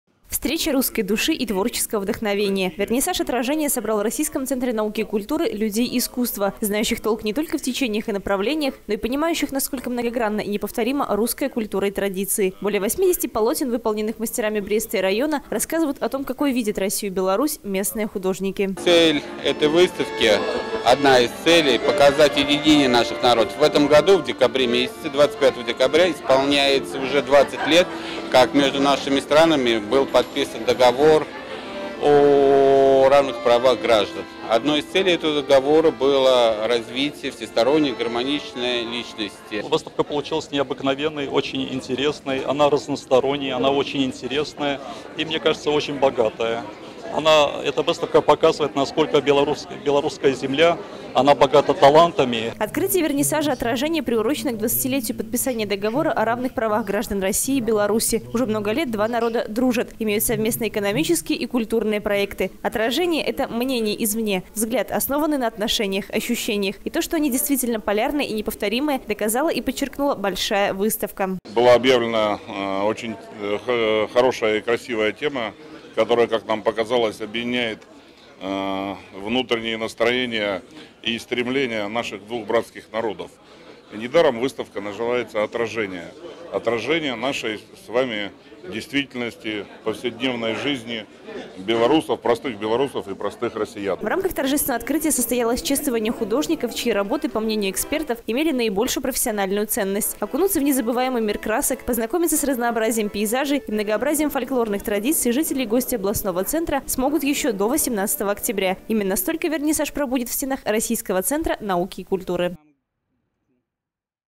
2018-09-28 г. Брест. Вернисаж «Отражение». Новости на Буг-ТВ.
О многообразии русского мира, его культурных традициях и уникальном колорите рассказывает выставка, открывшаяся накануне в стенах российского центра науки и культуры. Название вернисажа «Отражение» имеет глубокое философское значение: авторы постарались показать не только внешнюю красоту Руси, но и ее душу. Анастасия Ноздрин – Плотницкая побывала на открытии экспозиции.